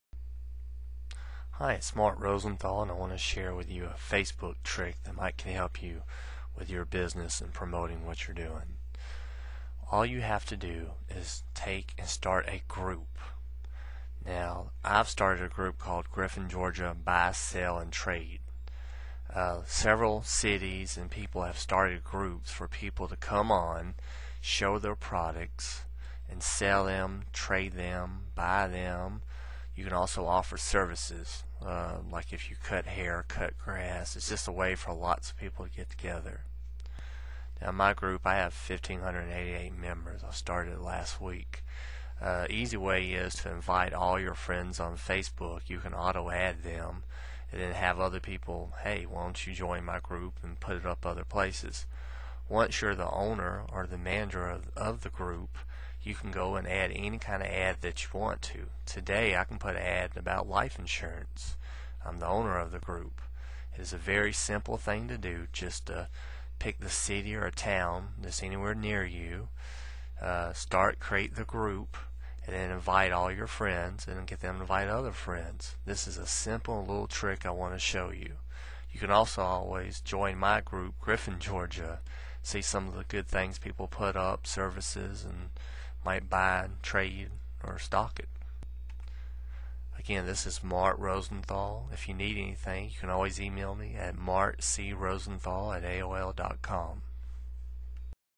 How making a facebook group can help you with your business
In this video, I will share with you something you can do on facebook to help promote your business for free.
This is very simple, easy and free to do.

You make this group local and are able to promote your services, business or products in the group your create. It is very easy to do. I started mine last week and already have over 1,500 members.

This is a fast growing thing that you need to do before someone else does it 1st in your area!

is my example of the group I created.